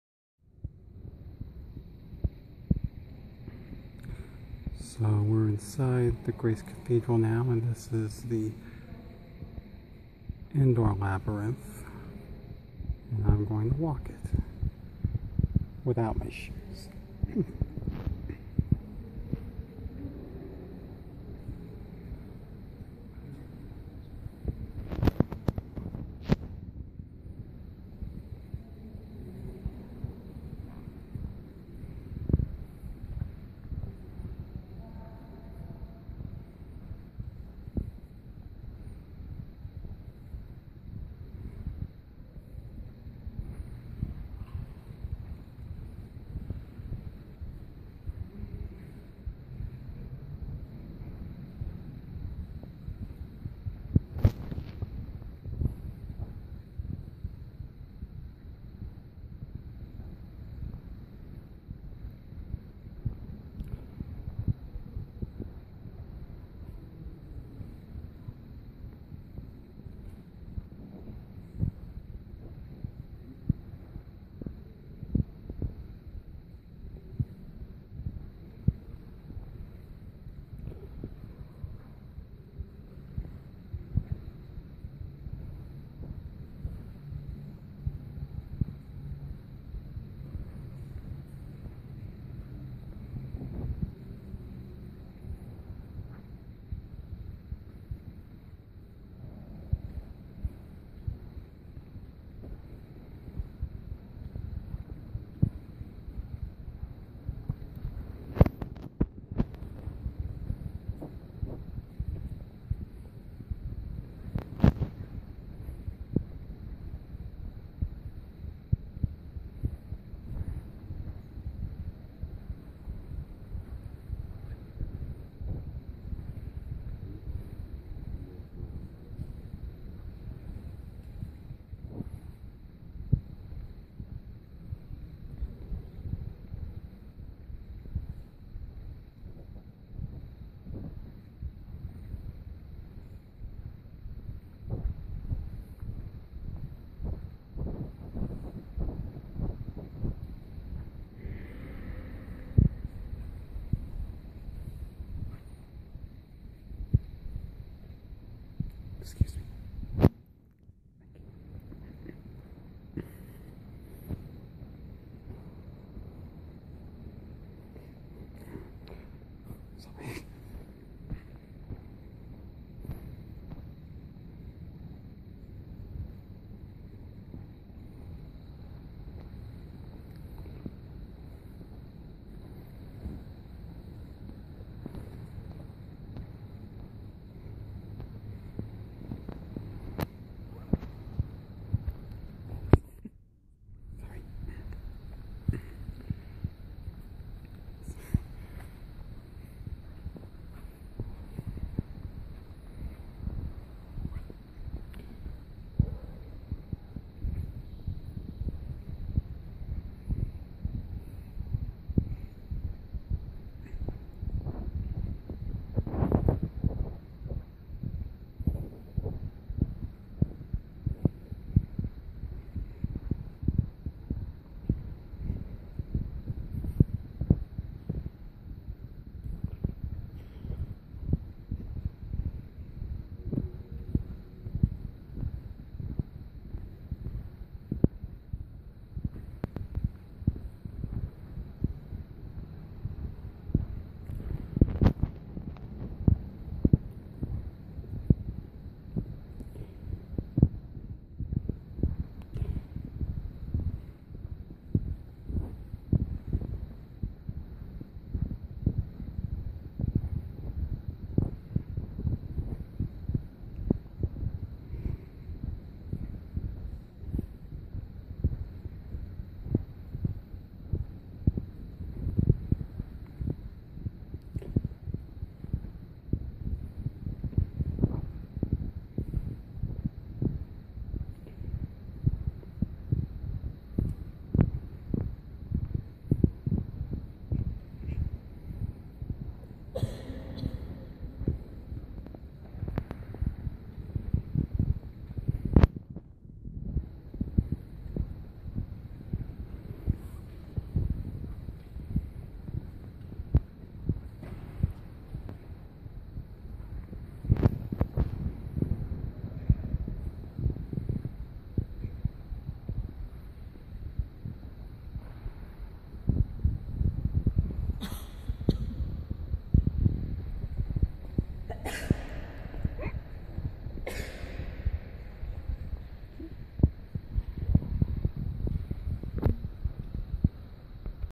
Indoor Labyrinth at Grace Cathedral
This is me walking through the indoor labyrinth at Grace Cathedral in San Francisco.